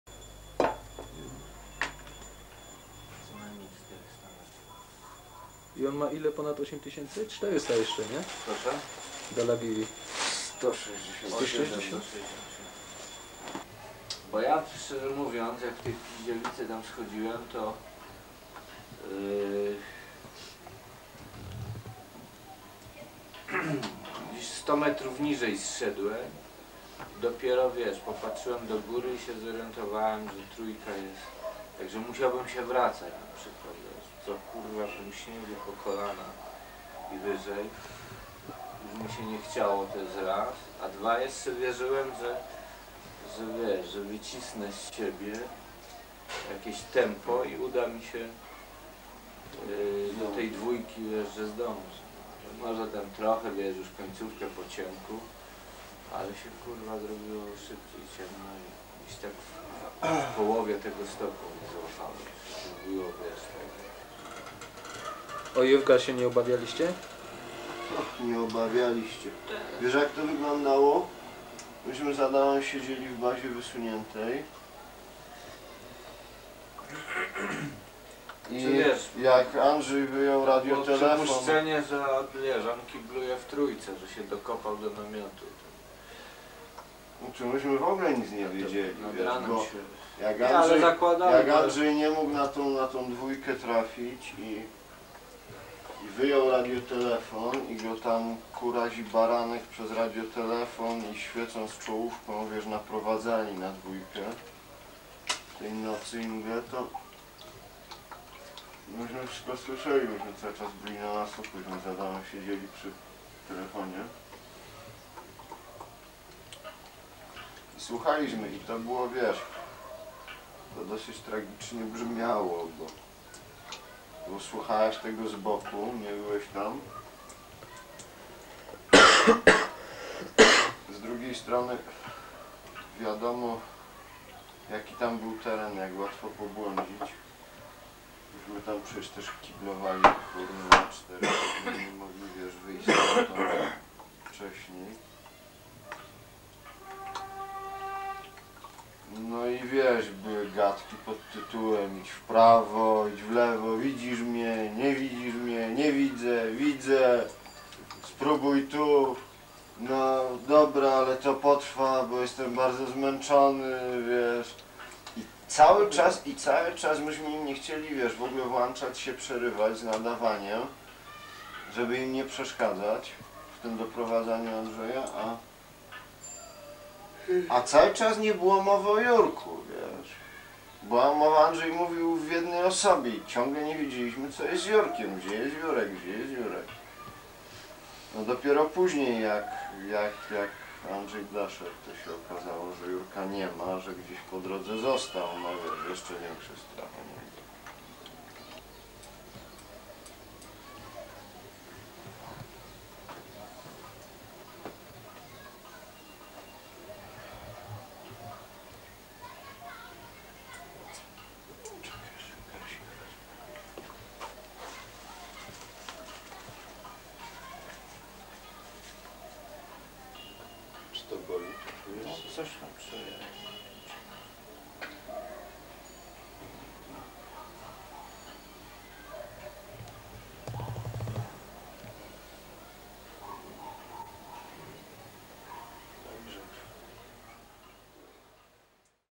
Odmrożenia, Andrzej Czok
Pielęgnacja nóg Andrzeja Czoka przez lekarza wyprawy. Dhaulagiri 1985, po powrocie z wyprawy, najprawdopodobniej w hotelu w Pokharze.

Sygnatura: 1005_02_B01_03V_M3_P1